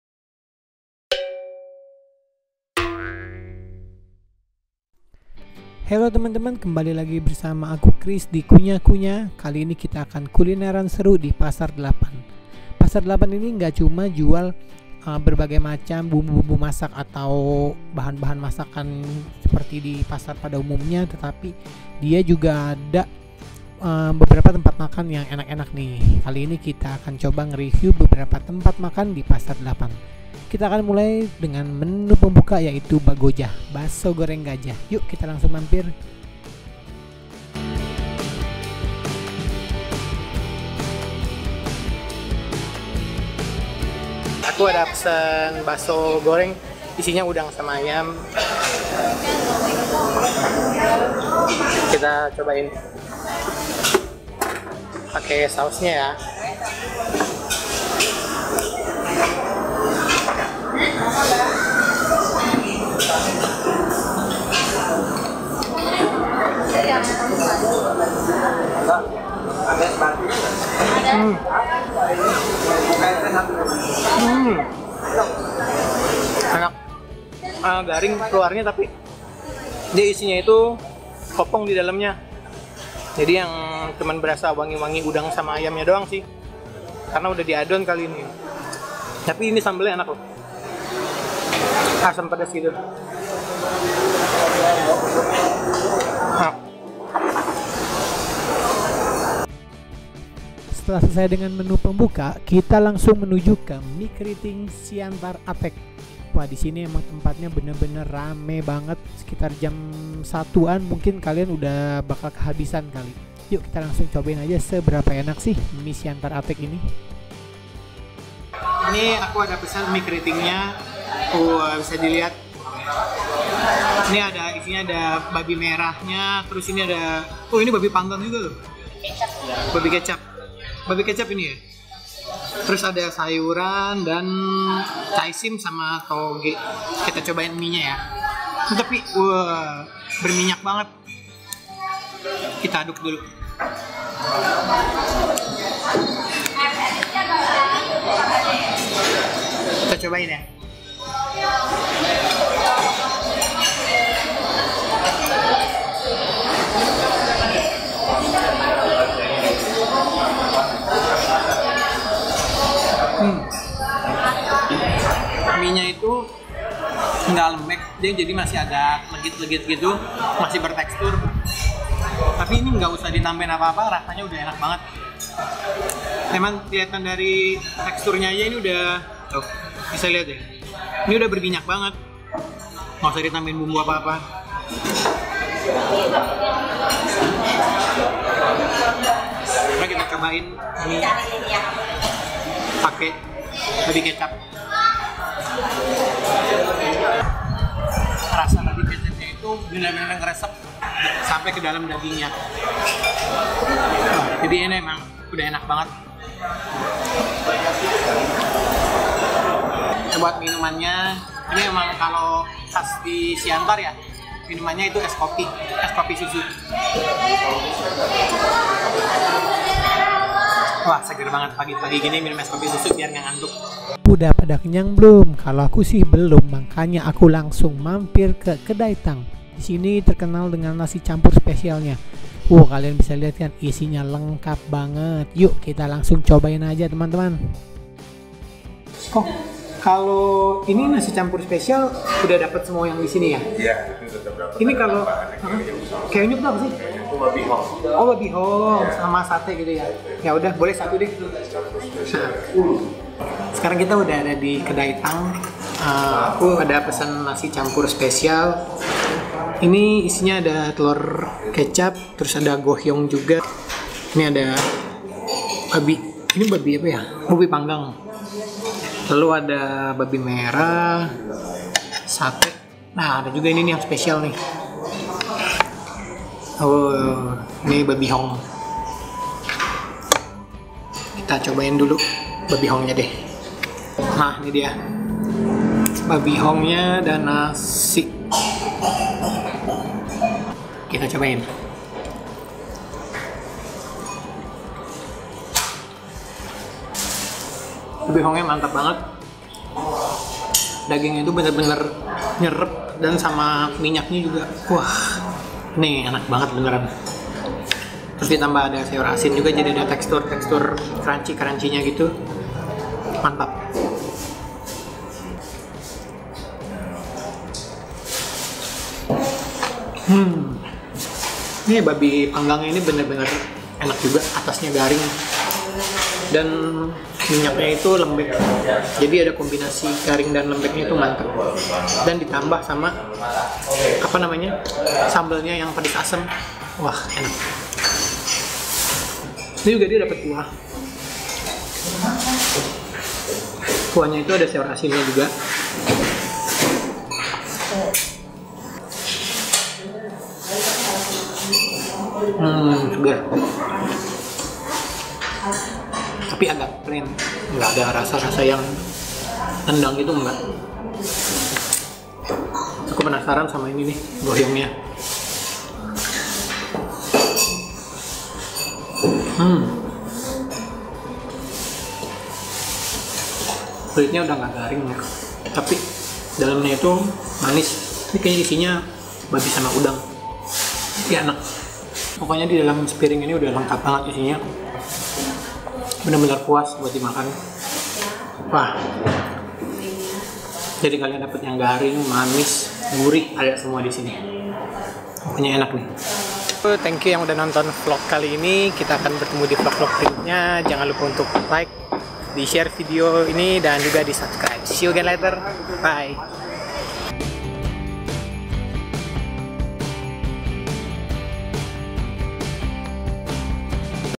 KULINER PAGI DI PASAR 8 ALAM SUTERA (ENAK-ENAK SEMUAAA)
Pasar 8 Alam  Sutera, BSD, Tangerang

Baso Goreng Gajah: 7/10
Bakmie Keriting Siantar Atek: 9/10
Nasi Campur Spesial Kedai Tang: 7.5/10